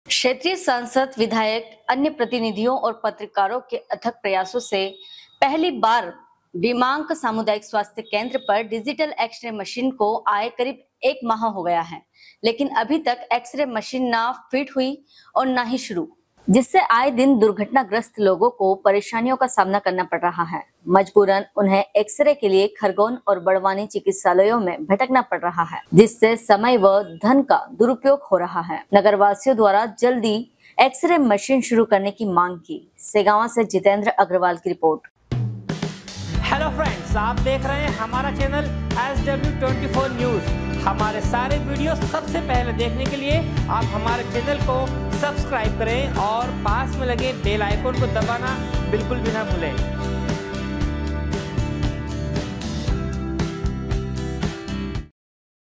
स्वास्थ्य केन्द्र पर करीब एक माह हो गया है। लेकिन अभी तक एक्सरे मशीन ना फीट हुई और नाही शुरू।
क्षैत्रिय सांसद विधायक, अन्य जनप्रतिनिधियो और पत्रकारों के अथक प्रयासों से पहली बार बीमांक सामुदायिक स्वास्थ्य केन्द्र पर डिजिटल एक्स-रे मशीन को आए करीब एक माह हो गया है। लेकिन अभी तक एक्सरे मशीन ना फीट हुई और नाही शुरू।
सेगांव से जितेन्द्र अग्रवाल की रिपोर्ट।